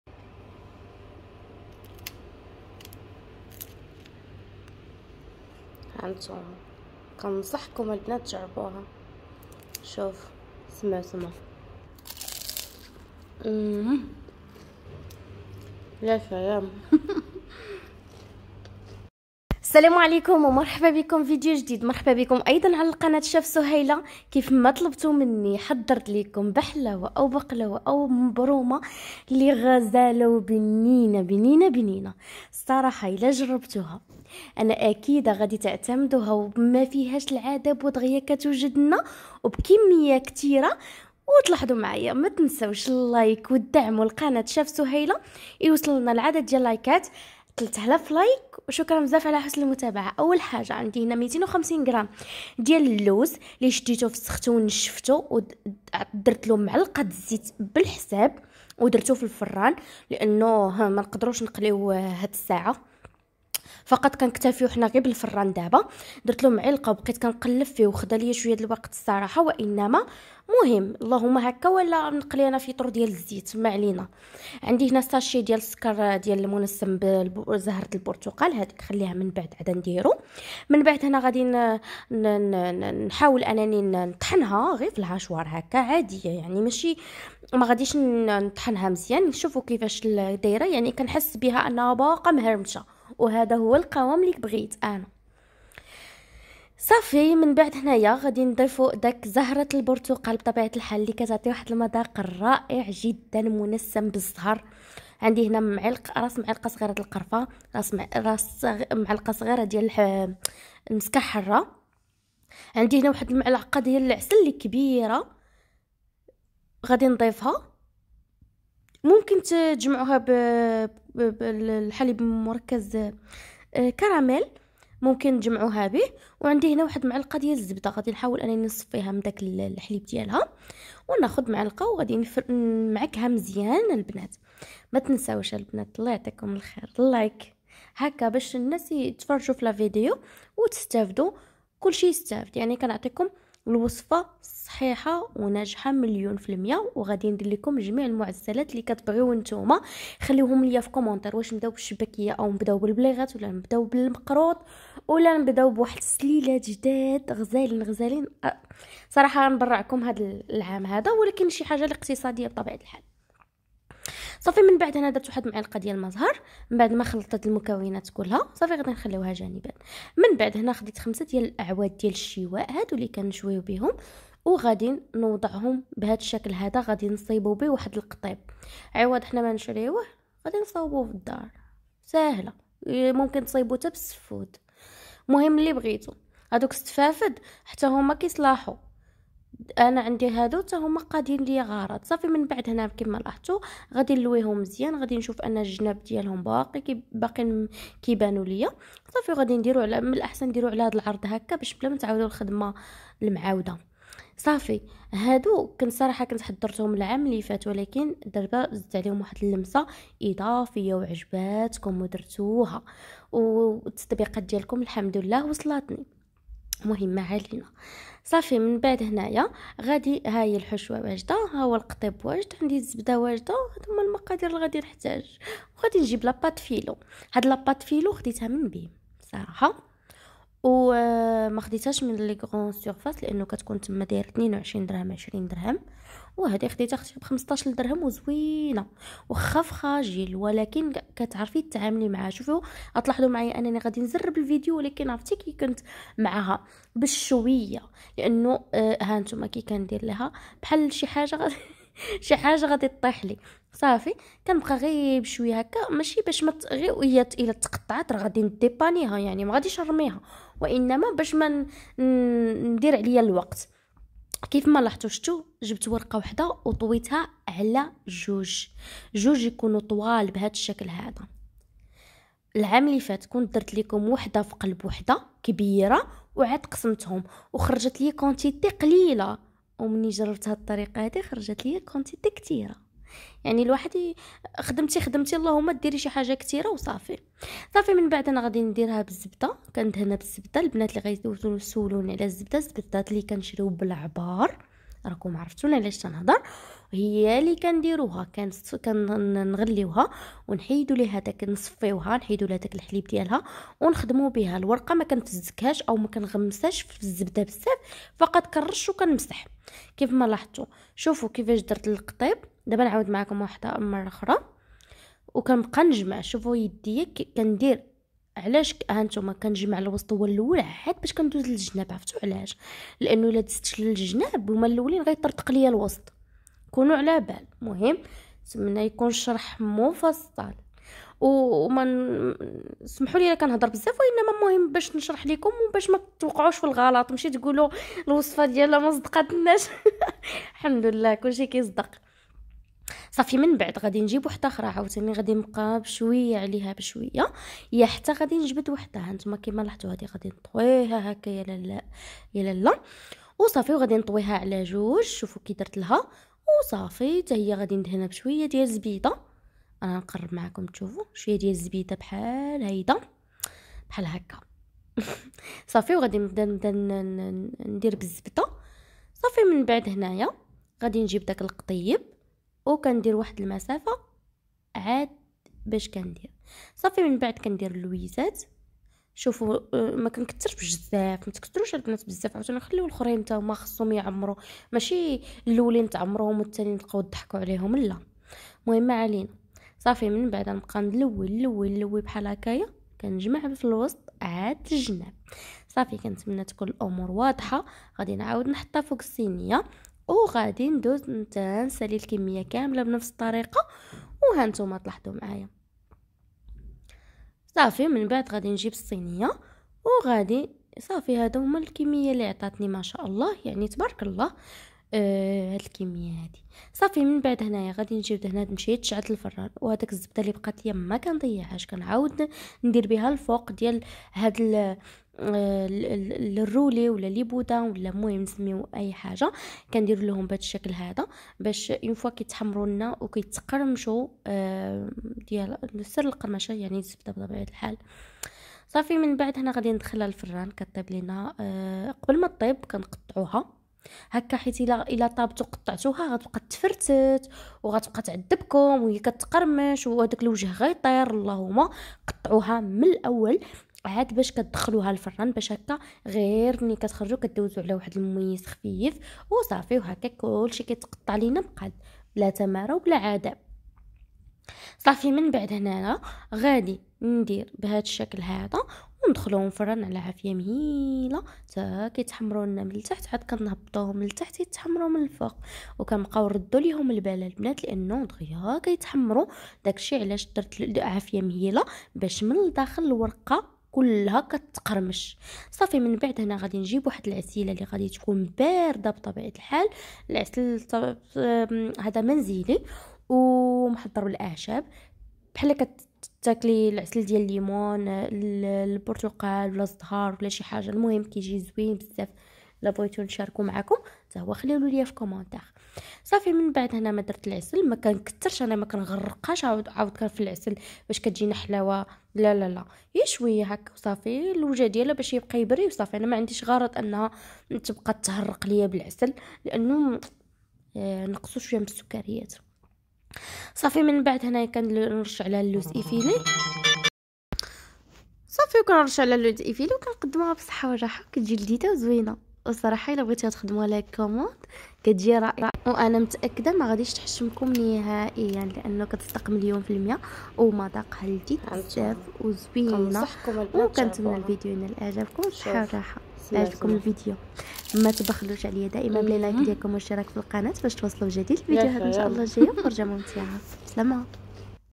بدون زيت جديد2022اسهل معسلة ممكن توجدي لرمضان بطريقة مختصرة سريعة بشكل غزال بحشوة تهبل بكمية كتيرة🇲🇦
ا للتعب🖐️حل لكل سيدة بدون مجهود اول مرة نخدم بقلاوة بهاد طريقة اكتر طلبا مخصراش فيكم بمذاق بلمسة
مقادير
رابعة لوز
قرفة
زهر
زبدة
عسل
ورقة الفيلو